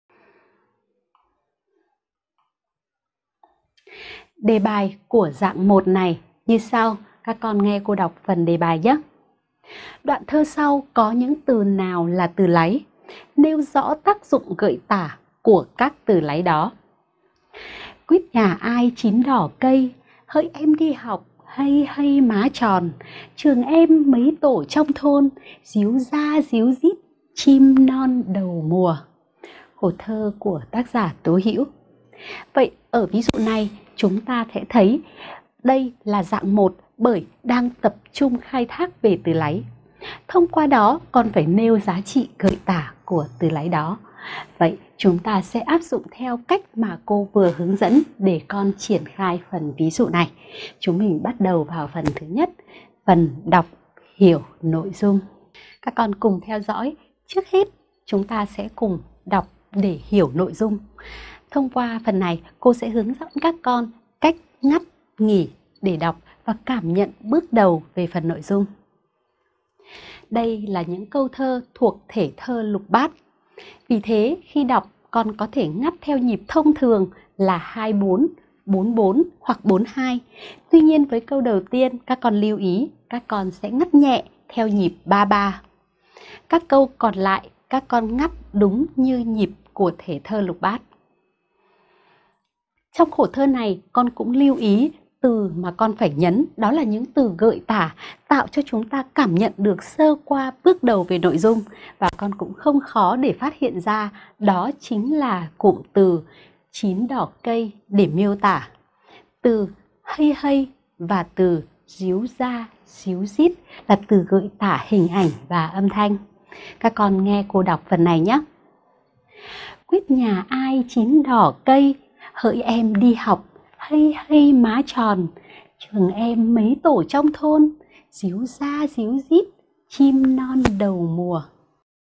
Từ láy – tác dụng gợi tả của từ láy (Xuân sớm) - Cảm thụ văn học 5
Học trực tuyến Cảm thụ văn học 5
Bài 1:
     Đoạn thơ dưới đây có những từ nào là từ láy? Hãy nêu rõ tác dụng gợi tả của các từ láy đó:
               Quýt nhà ai chín đỏ cây,
          Hỡi em đi học hây hây má tròn
        Trường em mấy tổ trong thôn
            Ríu ra ríu rít chim non đầu mùa.
                                 (Tố Hữu)
TỪ LÁY – TÁC DỤNG GỢI TẢ CỦA TỪ LÁY (XUÂN SỚM)